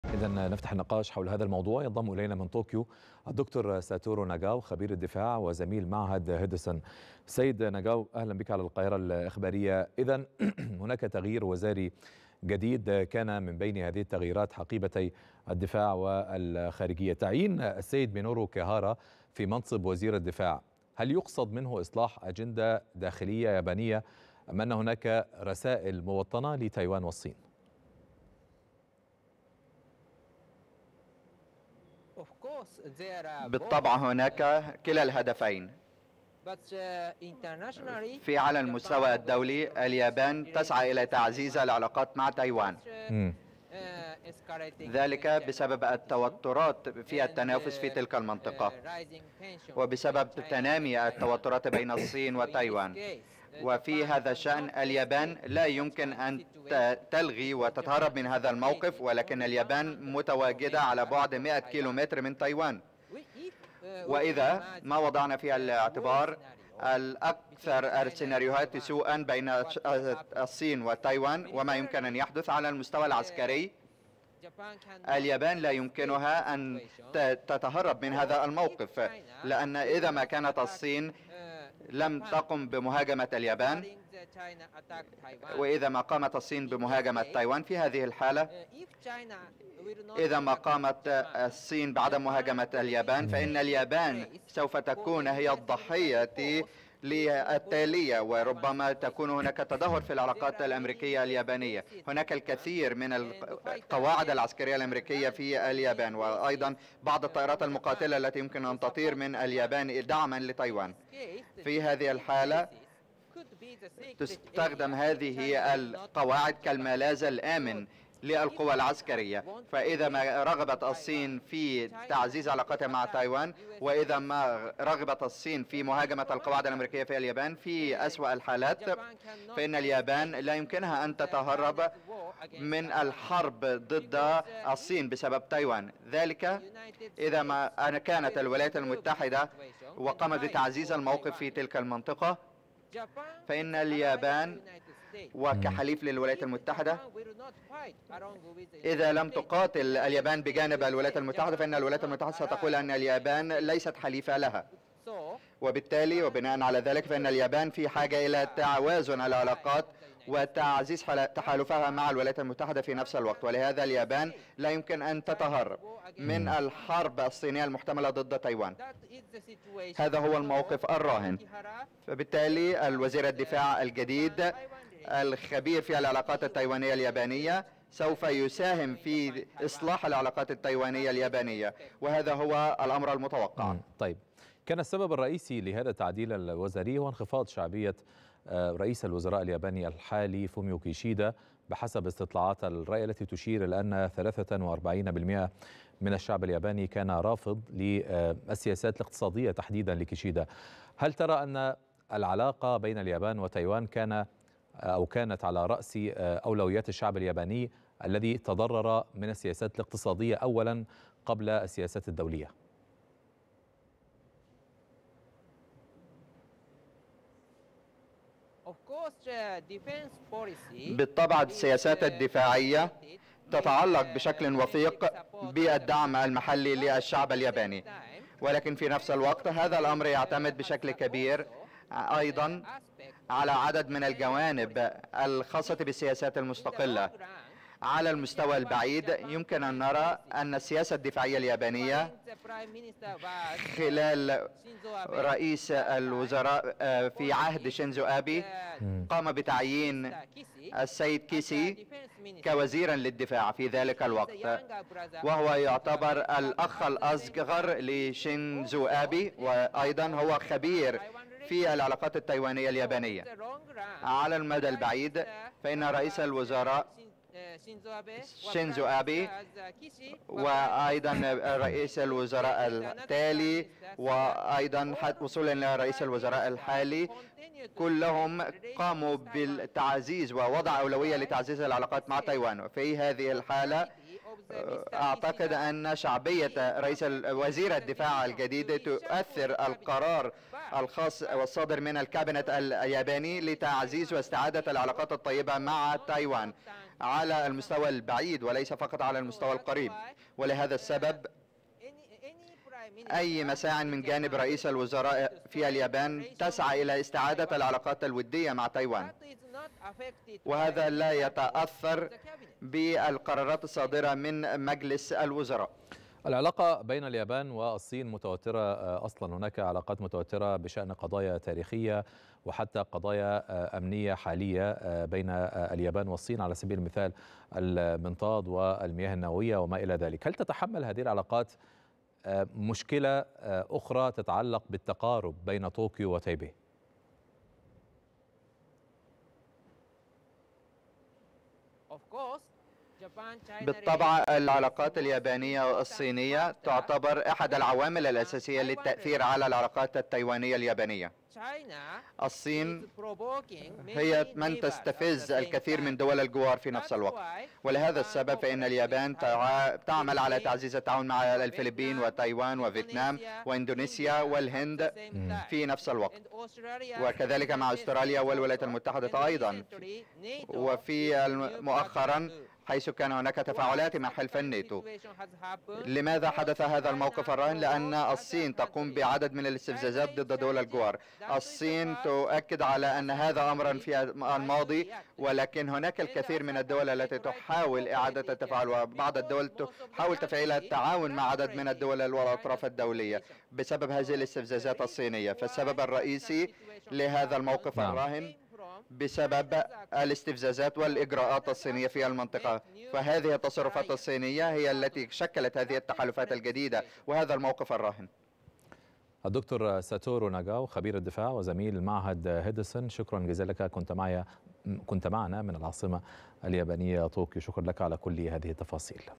د.ساتورو ناجاو زميل معهد هدسون: اليابان لا يمكن أن تتهرب من مهاجمة الصين في حال هجومها على تايوان
د.ساتورو ناجاو زميل معهد هدسون بطوكيو: اليابان لا يمكن أن تتهرب من مهاجمة الصين في  حال هجومها على تايوان

17/9/2023